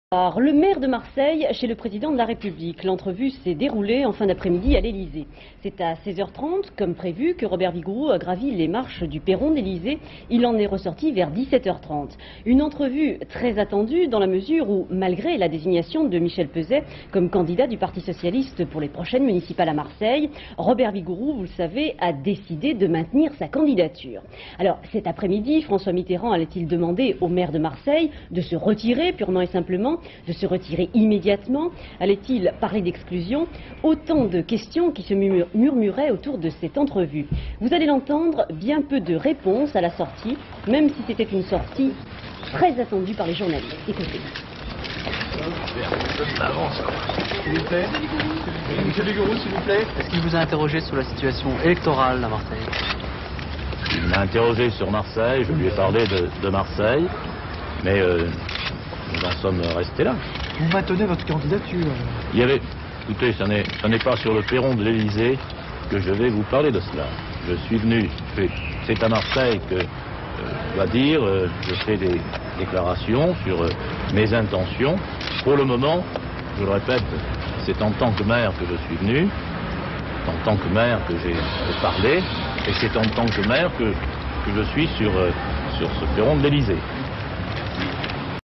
Interview de Robert VIGOUROUX
21 novembre 1988
Cette conférence de presse présente Robert VIGOUROUX interrogé sur les perrons de l'Elysée à propos de son entretien avec le président de la République.Il refuse de répondre s'il se retire du scrutin des élections municipales de Marseille  et déclare "je suis venu en tant que maire pour parler de Marseille" politique; archive television; archive tv; ina; inna; Institut National de l'Audiovisuel; french tv Images d'archive INA